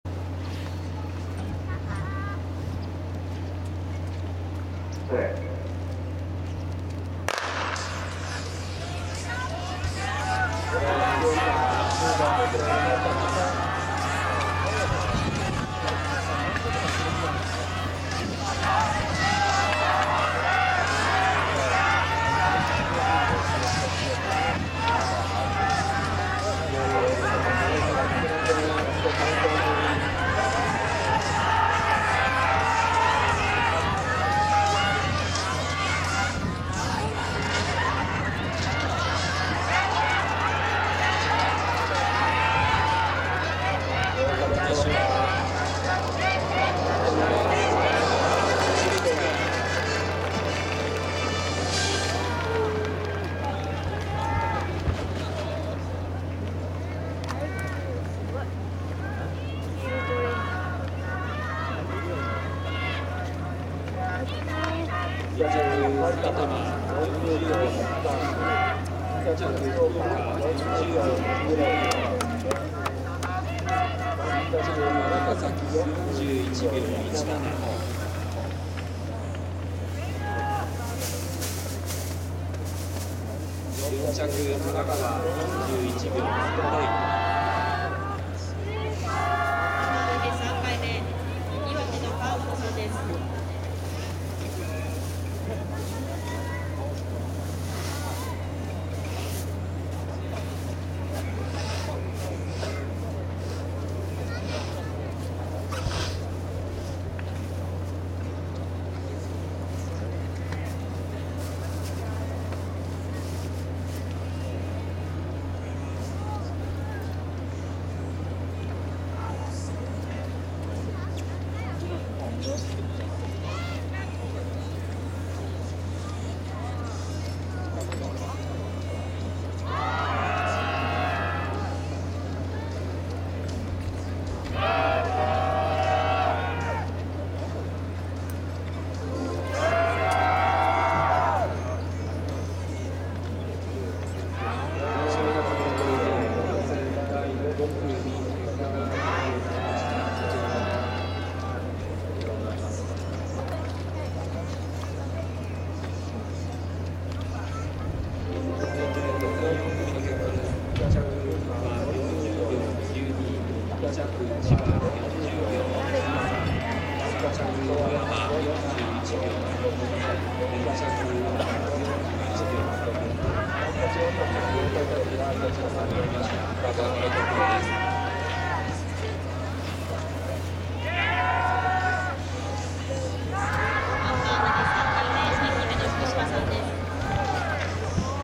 H28 いわて国体 男子4x100mR 予選5組
成年少年男子共通 4×100m 予選5組

1.40.83 Q　埼玉 （筧田-竹田-佐野-藤光）
2.40.90 Q　福岡 （木下-大瀬戸-竹下-南山）
3.41.17 q　長崎 （北川-永田-池田-尾脇）
4.41.30 q　神奈川 （岩崎-谷口-根本-山本）
5.41.37　栃木 （中里-落合-前原-高内）
6.41.58　島根 （花田-吾郷-畑中-門脇）
7.41.90　兵庫 （道下-小倉-溝口-小西）